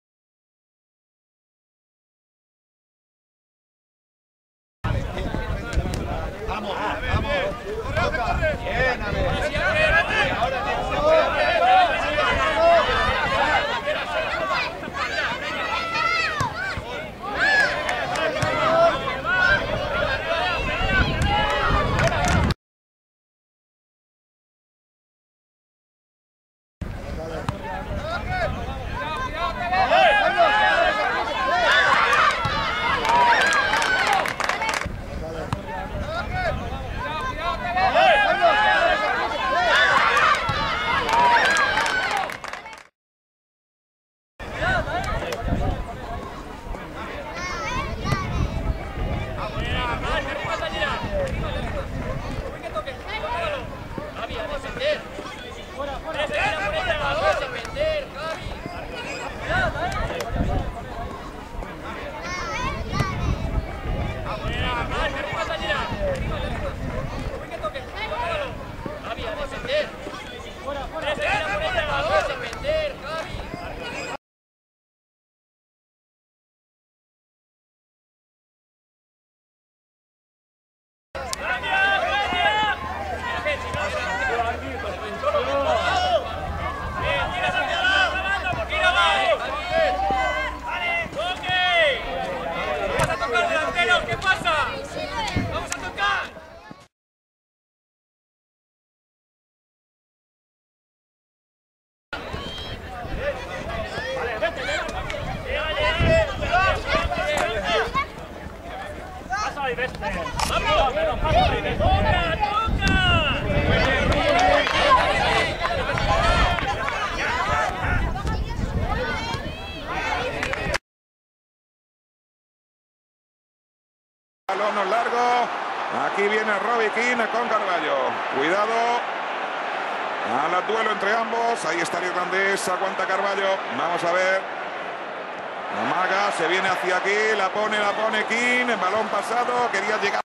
Planter_Benjamin-A_DEFENSA
Video empleado para la enseñanaza de fundamentos teórico-tácticos del Fútbol. Creado por la Escuela de Fútbol El Planter, Puzol, Valencia.